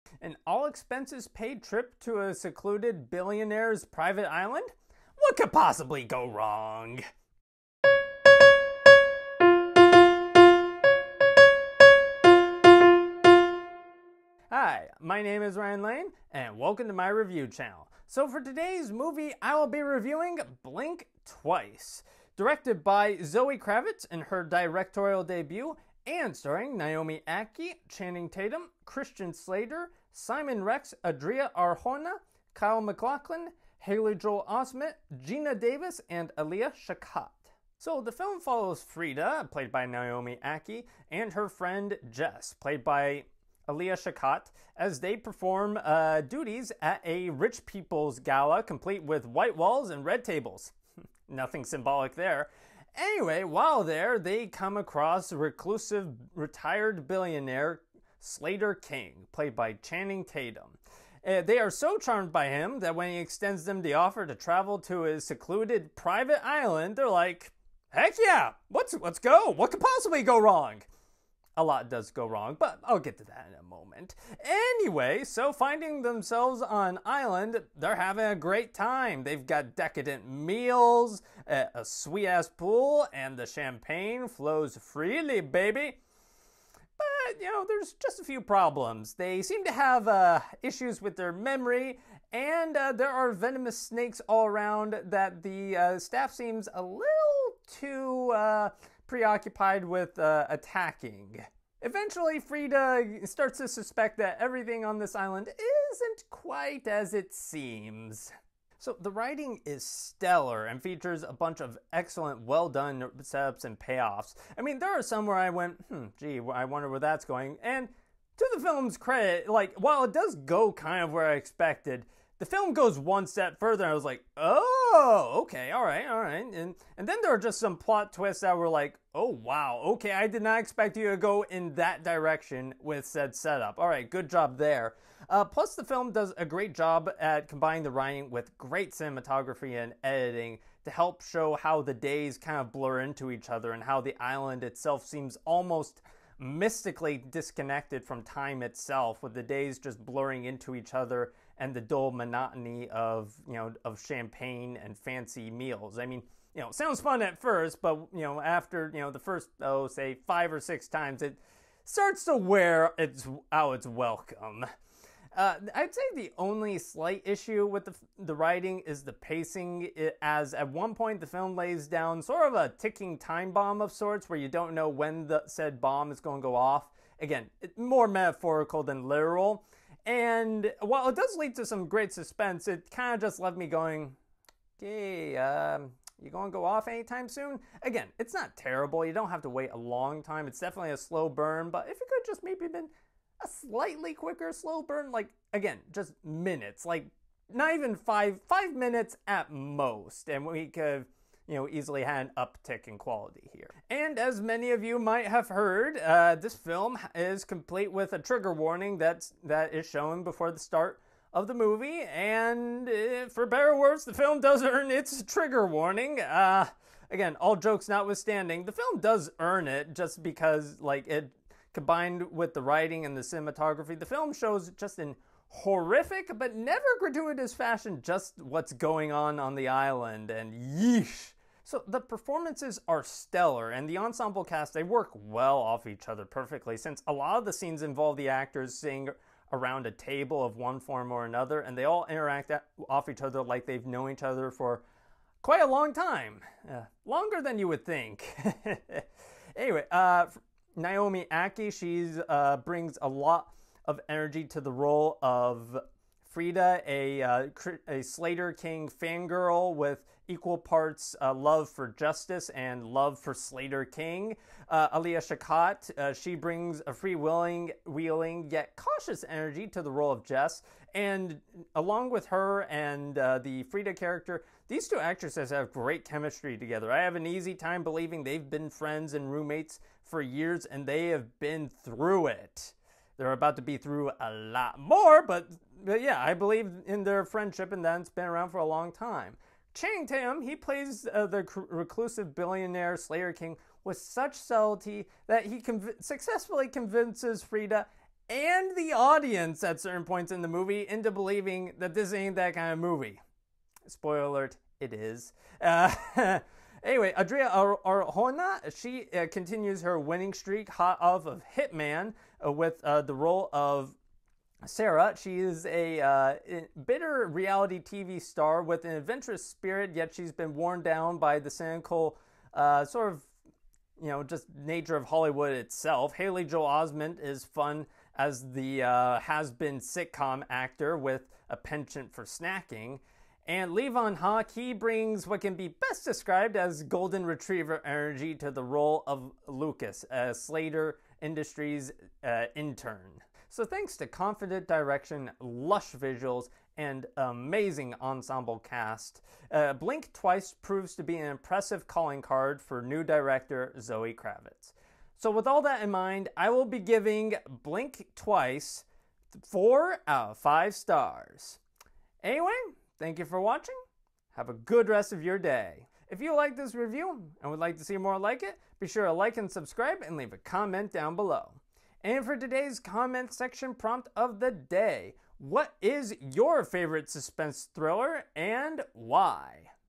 The dude from The Sixth Sense is in this! | Blink Twice Review
Directed By: Zoë Kravitz

Starring: Naomi Ackie, Channing Tatum, Christian Slater, Simon Rex, Adria Arjona, Kyle MacLachlan, Haley Joel Osment, Geena Davis, Alia Shawkat

Distributed By: Metro-Goldywn-Mayer

Released: August 23rd, 2024